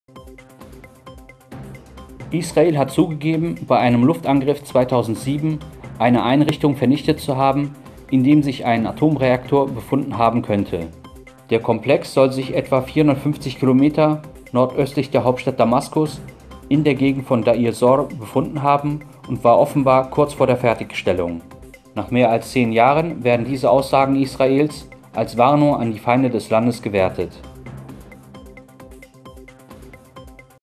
ISRAEL BESTÄTIGT NACH 10 JAHREN ANGRIFF AUF REAKTOR
Israel hat zugegeben, bei einem Luftangriff 2007 eine Einrichtung vernichtet zu haben, in dem sich ein Atomreaktor befunden haben könnte. Der Komplex soll sich etwa 450 km nordöstlich der Hauptstadt Damaskus in der Gegend von Deir-Zor befunden haben und war offenbar kurz vor der Fertigstellung. Nach mehr als 10 Jahren werden diese Aussagen Israels als Warnung an die Feinde des Landes gewertet.